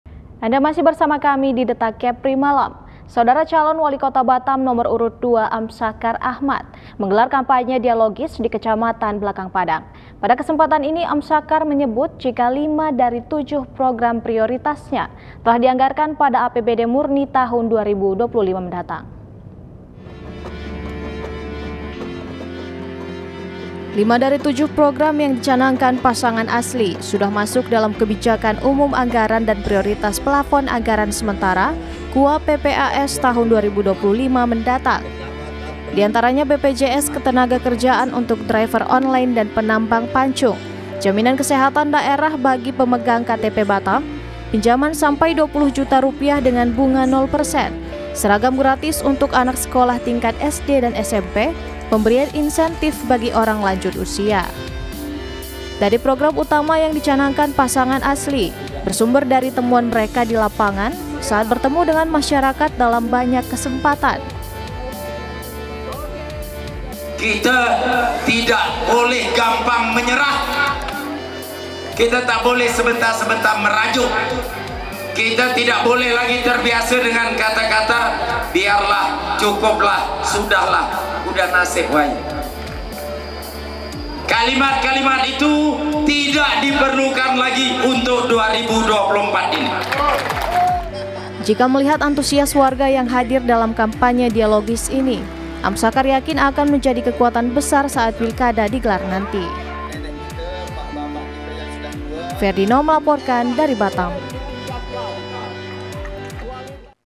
PROGRAM UNGGULAN ASLI JADI PEDOMAN APBD TAHUN 2025
Batamtv
 Calon Walikota Batam nomor urut dua Amsakar Ahmad, menggelar kampanye dialogis di Kecamatan Belakang Padang. Pada kesempatan ini Amsakar menyebut, jika 5 dari tujuh program prioritasnya telah dianggarkan pada APBD murni tahun 2025 mendatang.